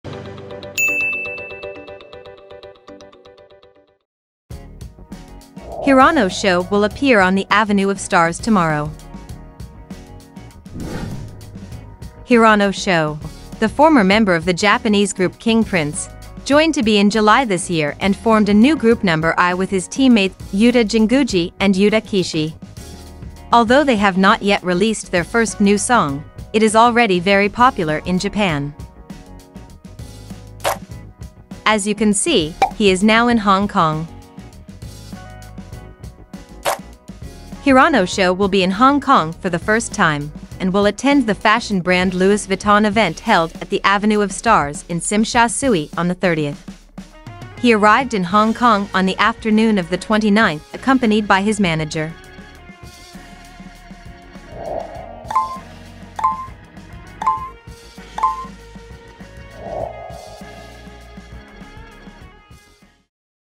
(Fashion Show) Hirano Sho Will Appear On The Avenue of Stars Tomorrow In Hong Kong
Hirano Sho, the former member of the Japanese group King & Prince, joined TOBE in July this year and formed a new group Number_i with his teammates Yuta Jinguji and Yuta Kishi.